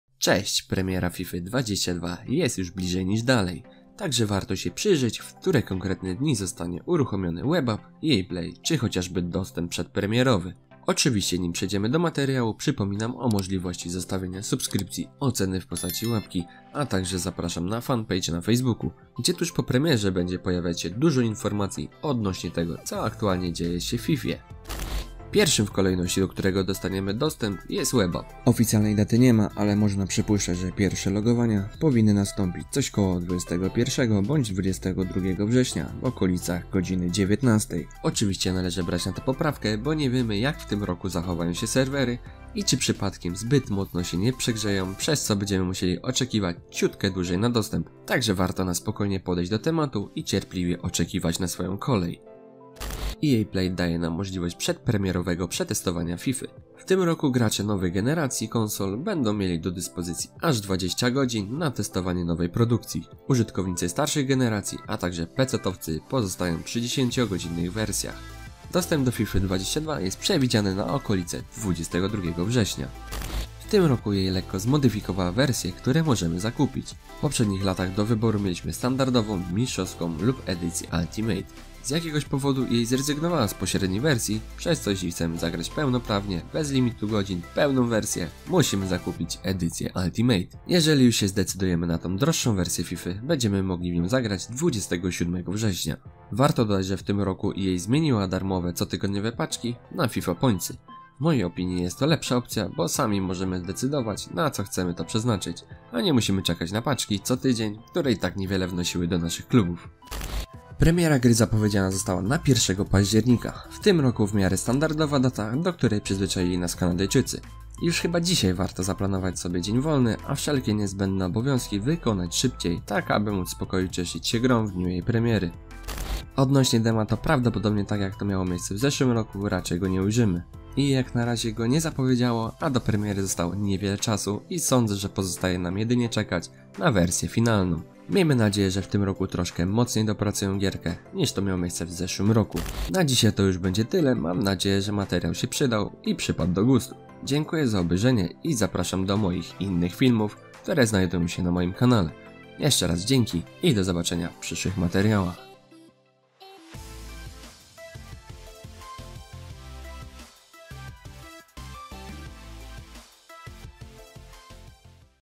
Fifa 22 Kiedy Web app, Ea play, Premiera?
W dzisiejszym filmie przyjrzymy się poszczególnym datom związanymi z fifa 22. Każdy z nas już czeka na pierwsze logowania do web appa, pierwszego meczu w próbnej wersji Ea play, czy chociażby na wersję finalną, której premiera zbliża się wielkimi krokami. Dlatego powstał ten materiał, aby na wszystkie te pytania odpowiedzieć i zaplanować sobie najbliższy czas tak, aby nic nie stanęło nam na przeszkodzie i cieszenia się z nowej odsłony fify!

👍 Zostaw Like'a!
💪Nie zapomnij o Subie!
🔔 Dzwoneczek będzie twoim dodatkowym atutem :D
⌨ Napisz swoją opinie w komentarzu!